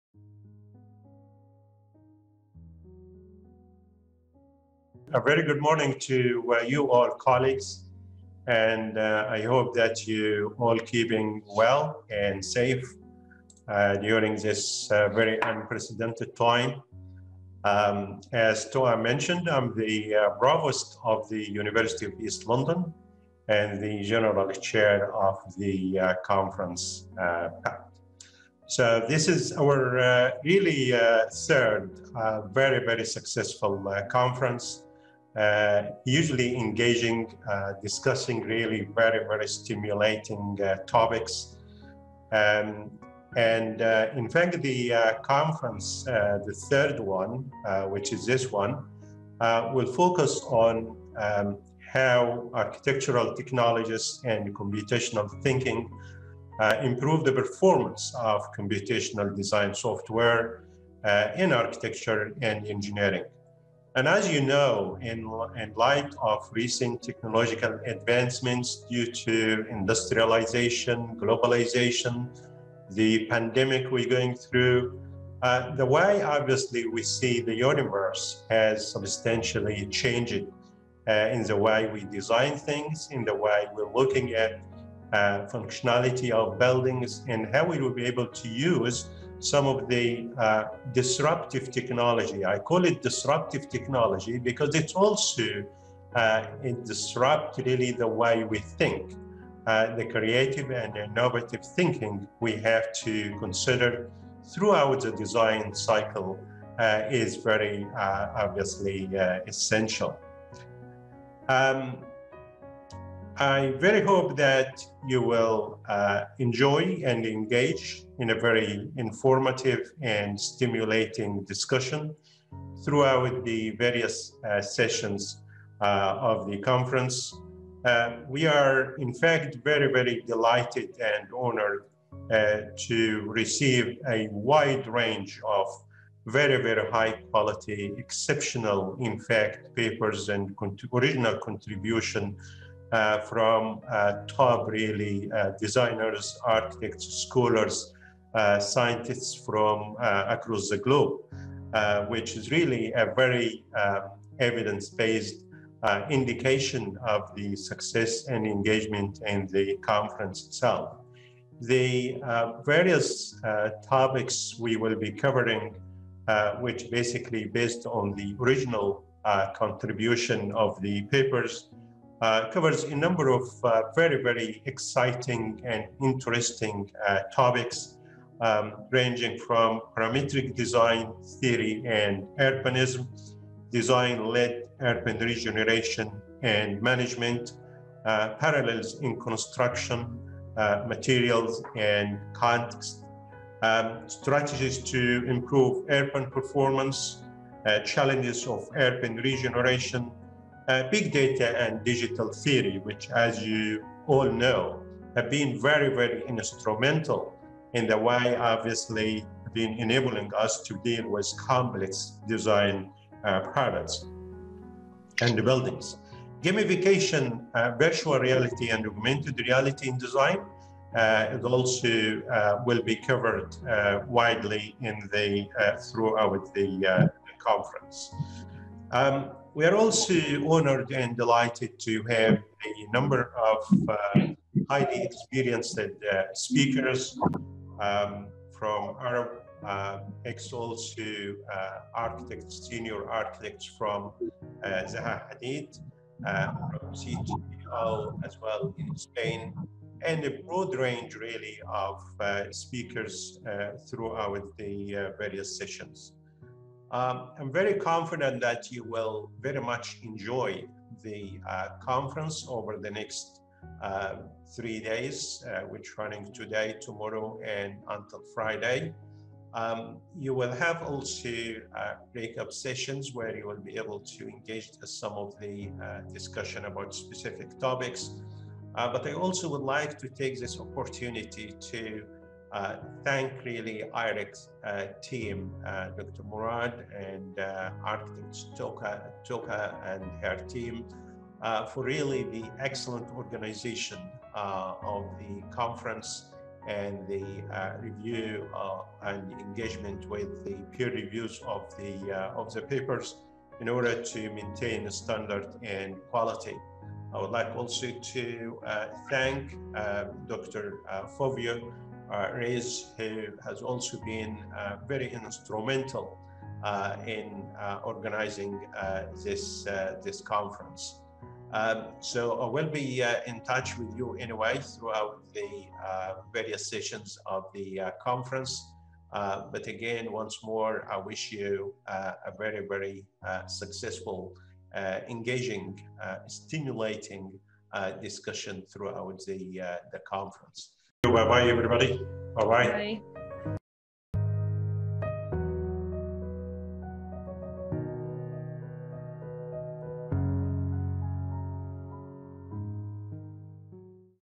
PACT 2020 | A Welcome from the Conference Chair - Prof. Hassan Abdalla
Consequent to the success of its previous editions in 2016 and 2018, the third edition of the ‘Parallelism in Architecture, Engineering & Computing Techniques’, organized in collaboration with the University of East London (UEL) in UK, was held online.

In this video, a brief overview of the conference topics, the contributons and organization is given by the conference chair, Prof. Hassan Abdalla, the Dean of the School of Architecture, Computing & Engineering in UEL, UK, during his welcoming speech.

The conference unveiled the latest trends of architectural technologies and computational thinking as a way of improving the performance of computational design software in architectural and engineering discourses. In a fast-paced world and in light of recent technological advancements due to industrialization and globalization, the way we see the universe has changed. That being said, the conference has shed a light on the ways in which parallelism can be integrated into different architectural and computing dialogues through stimulating a discussion on topics of Complex Digital Fabrication, Augmented Reality, Intelligent Environments, Advanced Fabrication Technologies, Big Data and more.